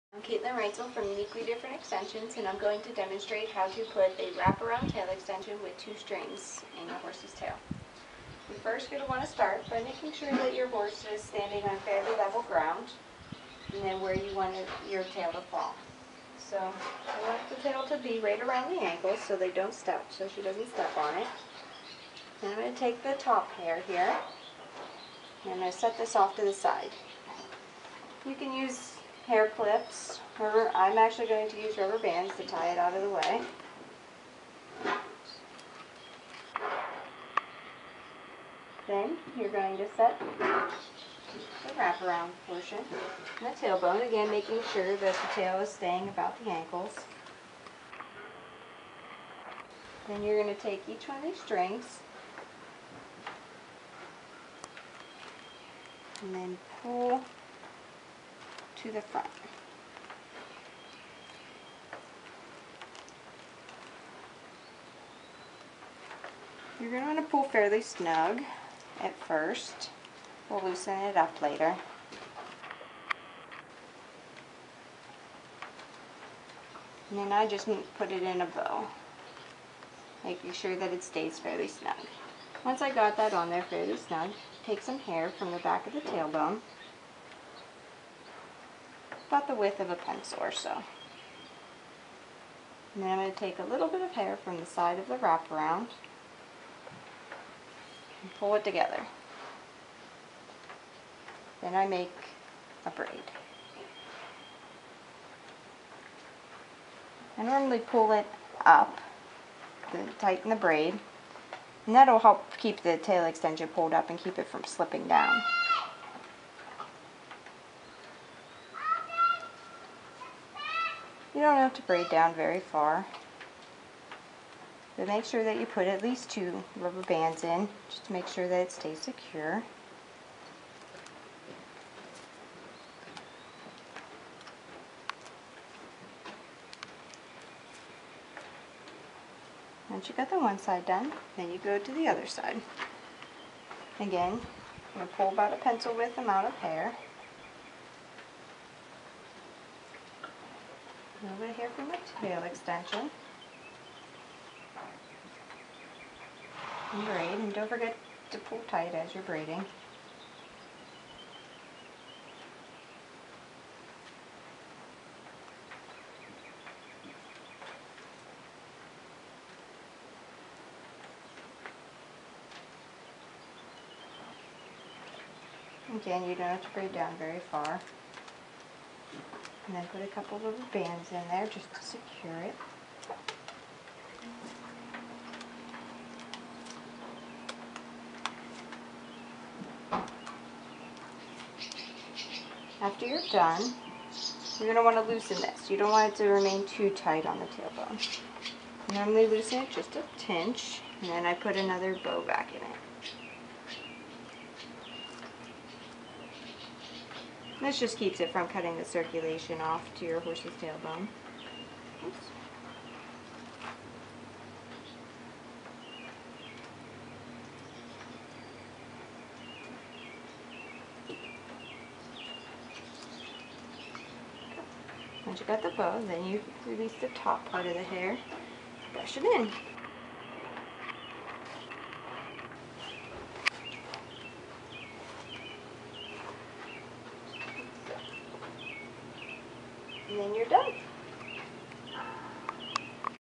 How to attach a wrap around tail extension with strings
How to attach a wrap around tail extension with two strings. By Uniquely Different Extensions.